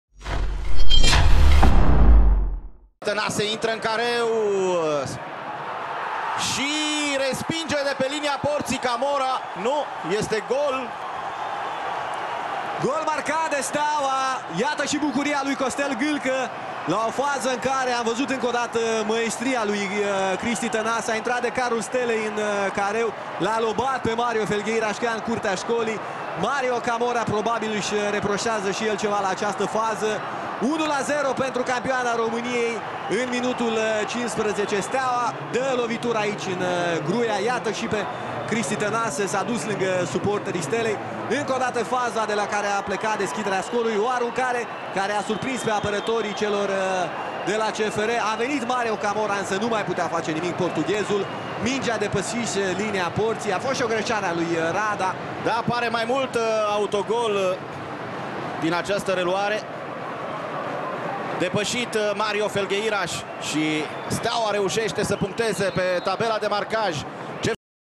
Tănase deschide scorul în Gruia! CFR - Steaua 0-1
Steaua a reuşit să deschidă scorul pe stadionul CFR-ului în minutul 16! Tănase a pătruns irezistibil în careul gazdelor şi a lobat peste Felgueiras, care s-a văzut obligat să scoată balonul din plasă.